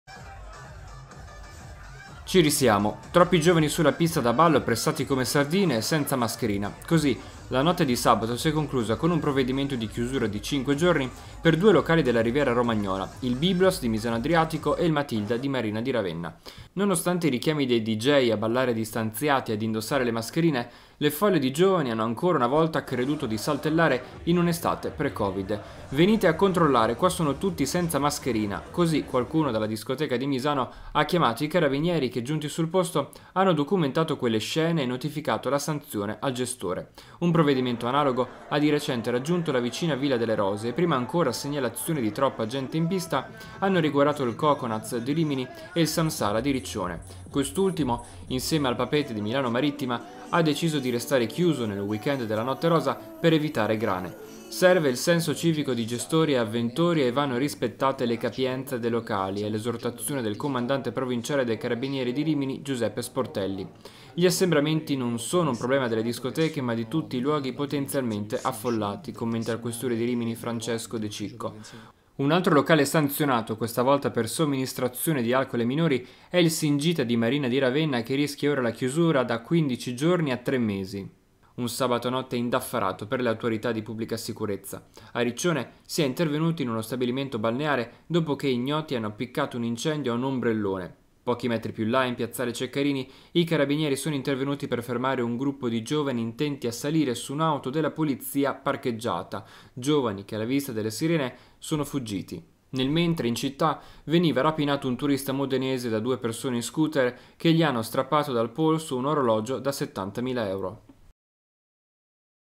ROMAGNA: Movida e assembramenti, chiusi locali Byblos e Matilda | VIDEO
Ci risiamo. Troppi giovani sulla pista da ballo pressati come sardine e senza mascherina. Così la notte di sabato si è conclusa con un provvedimento di chiusura di 5 giorni per due locali della Riviera romagnola: il Byblos di Misano Adriatico e il Matilda di Marina di Ravenna. Nonostante i richiami dei dj a ballare distanziati e ad indossare le mascherine, le folle di giovani hanno ancora una volta creduto di saltellare in un’estate pre-Covid. “Venite a controllare, qua sono tutti senza mascherina”. Così qualcuno dalla discoteca di Misano ha chiamato i carabinieri che, giunti sul posto, hanno documentato quelle scene e notificato la sanzione al gestore. Un provvedimento analogo ha di recente raggiunto la vicina ‘Villa delle Rose’, e prima ancora segnalazioni di troppa gente in pista hanno riguardato il ‘Coconuts’ di Rimini e il ‘Samsara’ di Riccione. Quest’ultimo, insieme al ‘Papeete’ di Milano Marittima, ha deciso di restare chiuso nel weekend della Notte Rosa per evitare grane. Serve il “senso civico di gestori e avventori” e “vanno rispettate le capienze” dei locali, è l’esortazione del comandante provinciale dei carabinieri di Rimini, Giuseppe Sportelli. “Gli assembramenti non sono un problema delle discoteche, ma di tutti i luoghi potenzialmente affollati”, commenta il questore di Rimini, Francesco De Cicco. Un altro locale sanzionato, questa volta per somministrazione di alcol ai minori, è il Singita di Marina di Ravenna che rischia ora la chiusura fino a 15 giorni. Un sabato notte indaffarato per le autorità di pubblica sicurezza. A Riccione si è intervenuti in uno stabilimento balneare dopo che ignoti hanno appiccato un incendio a un ombrellone. Pochi metri più in là in piazzale Ceccarini i carabinieri sono intervenuti per fermare un gruppo di giovani intenti a salire su un’auto della polizia parcheggiata. Giovani che alla vista delle sirene sono fuggiti. Nel mentre in città, un turista modenese è stato rapinato da due persone in scooter che gli hanno strappato dal polso un orologio da 70 mila euro.